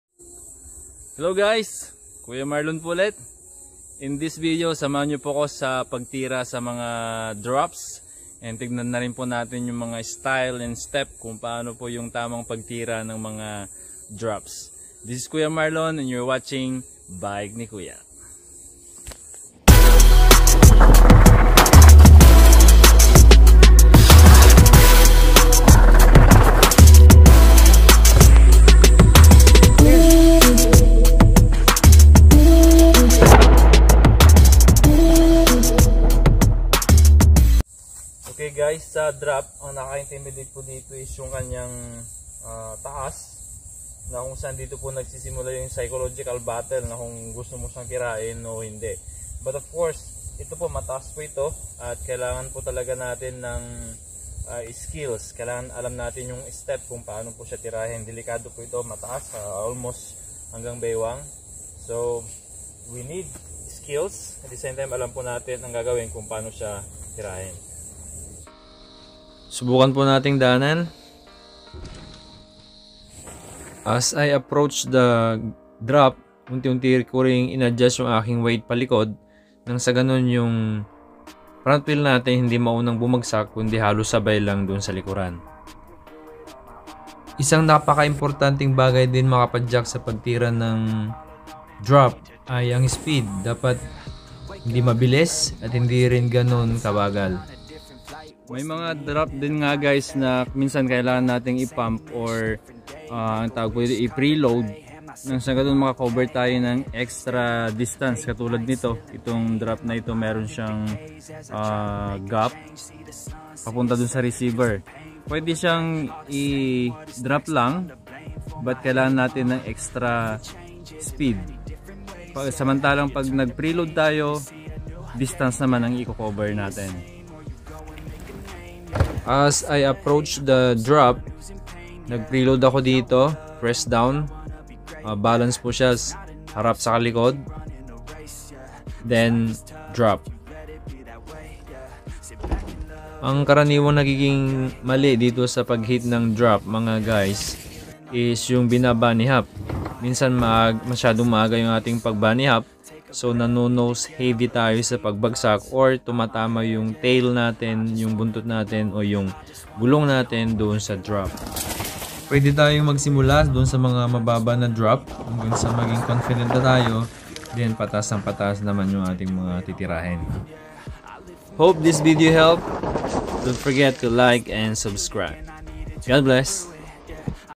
How to drop MTB
In this video I would like to teach you on how to do drop on a MTB. I will give tips on how to do it right. Start practicing now and run all the drop in your local trail.